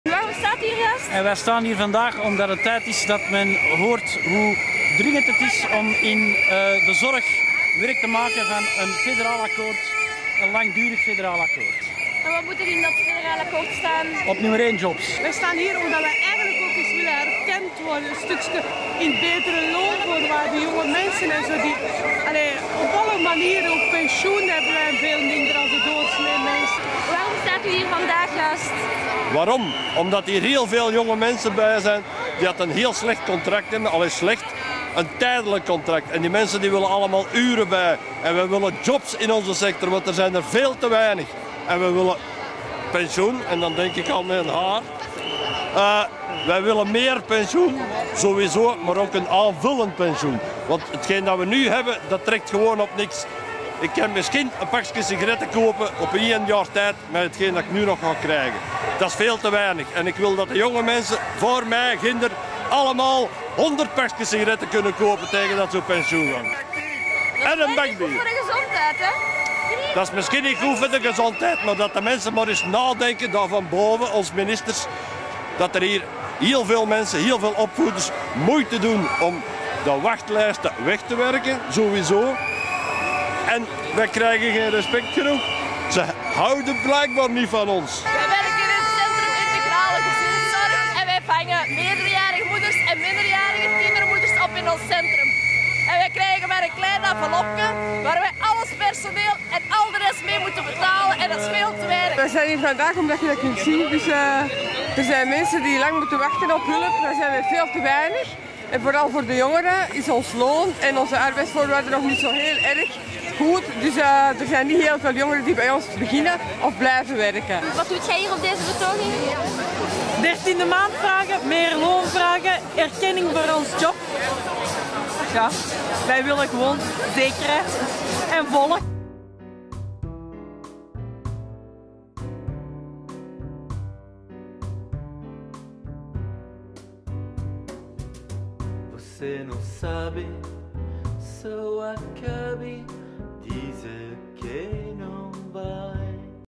Waarom u op straat kwam
De non-profit betoogde en Antwerpen heeft het geweten. Wittewoede.be stuurde reporter Lisa om te weten te komen waarom u betoogde.